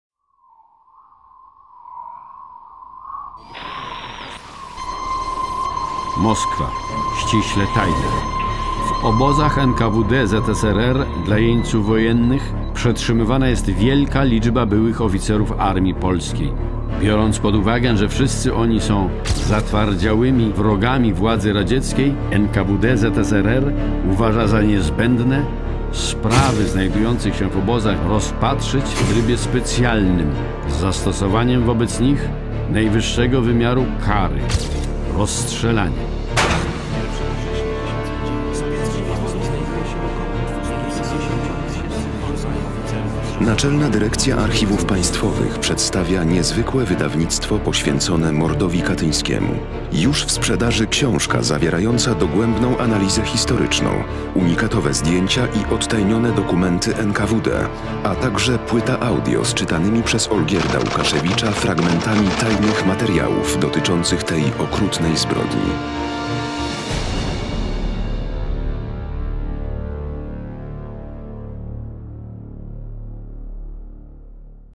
Mord Katyński - trailer publikacji Naczelnej Dyrekcji Archiwów Państwowych (HD)
Film promuje 2 publikacje wydane nakładem Naczelnej Dyrekcji Archiwów Państwowych - książkę popularno-naukową prof. Wojciecha Materskiego pt. "Mord katyński. Siedemdziesiąt lat drogi do prawdy" oraz audiobooka pt. "Mord katyński. Dokumenty zbrodni".

Książka jest kompendium wiedzy o siedemdziesięcioletnim procesie dochodzenia do prawdy o bezprecedensowej zbrodni katyńskiej. Bazą dla wydawnictwa stała się obszerna dokumentacja źródłowa pochodząca z archiwów polskich i rosyjskich oraz literatura przedmiotu.

Audiobook stanowi wybór kilkudziesięciu dokumentów wraz z komentarzami, dokonany spośród ponad ośmiuset znajdujących się w czterotomowej publikacji poświęconej zbrodni katyńskiej („Katyń. Dokumenty zbrodni"), która ukazała się staraniem Naczelnej Dyrekcji Archiwów Państwowych w latach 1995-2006.
Czytają: Olgierd Łukaszewicz i Jacek Rozenek